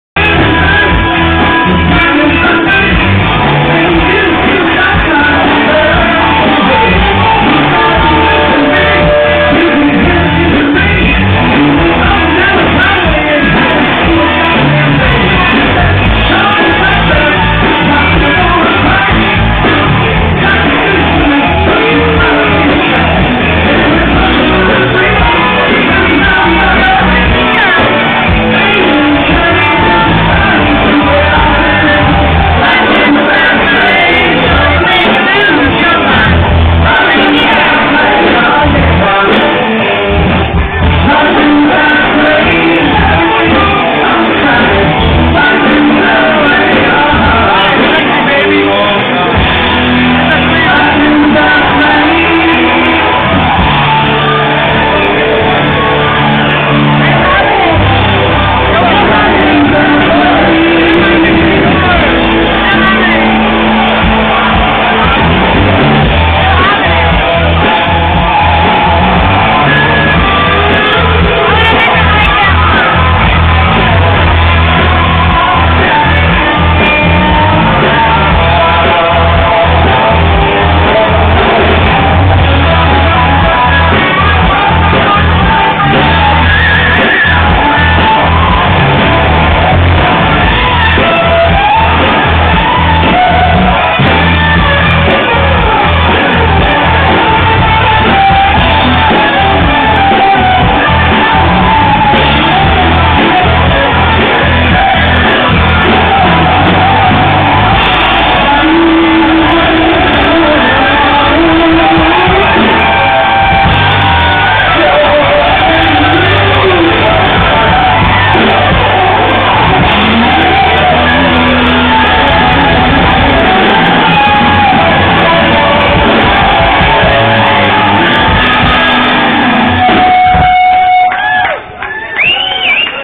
mallory smoking hot
hot chick smoking a cigar on bouron street